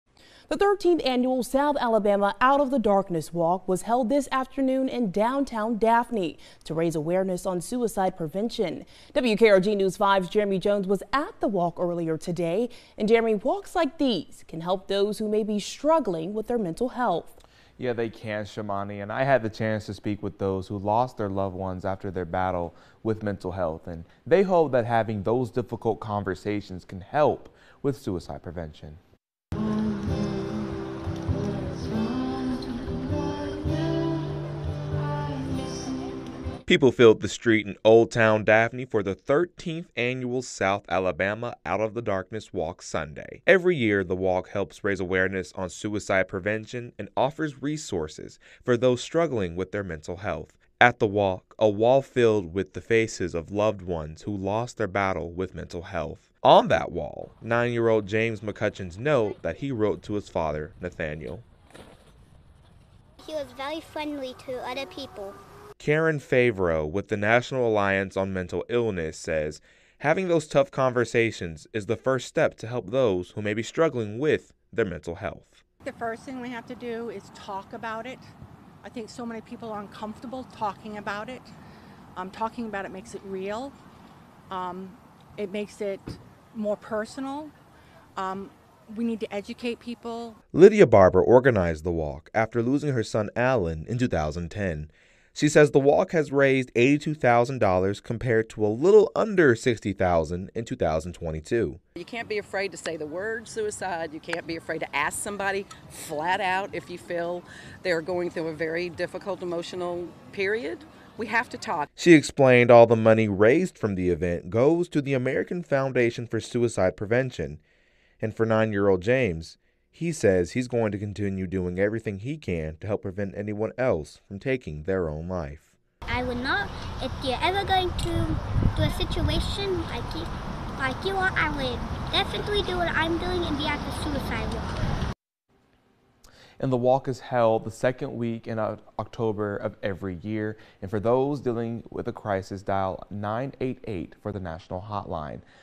'Out of Darkness' Walk in Daphne aims at suicide prevention
People filled the streets in Downtown Daphne for the 13th annual "Out of Darkness" Awareness Walk. They were there to raise awareness of important conversations about mental health that could prevent self-harm.